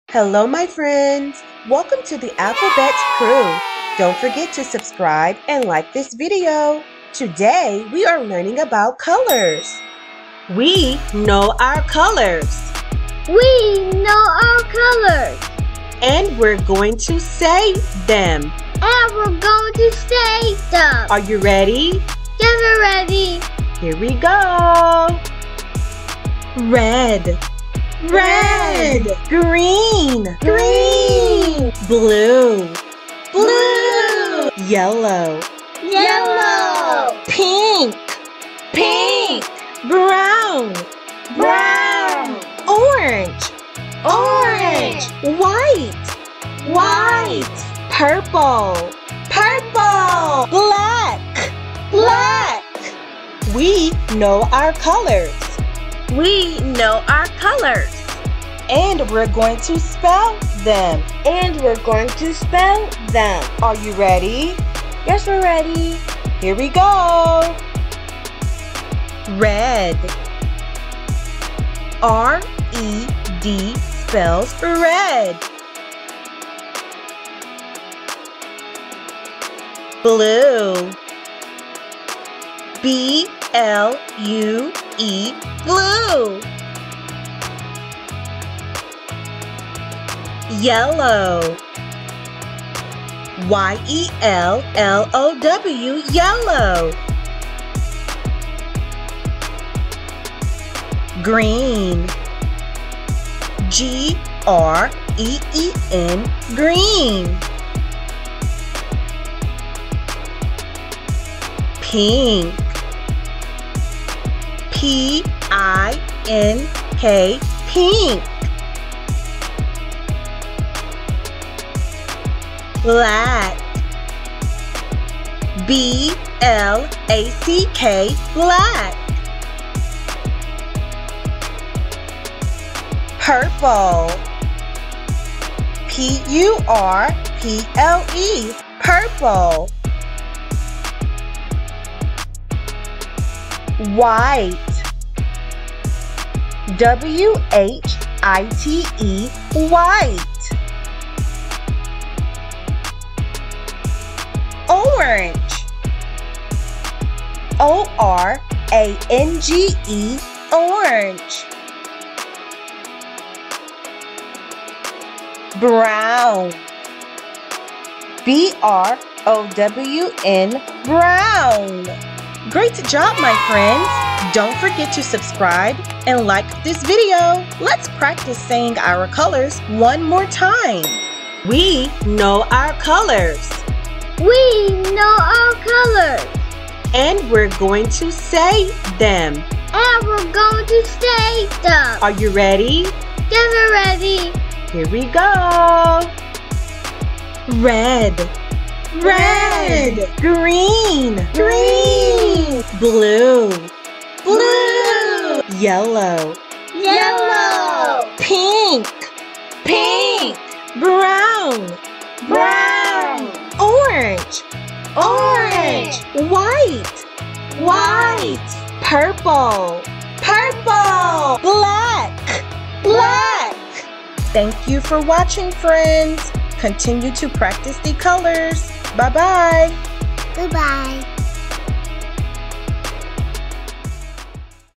Brighten Up Your Day! Learning Colors With A Fun Song
Let's Learn colors with the Alphabet Crew! This toddler learning video teaches colors in a fun and engaging song!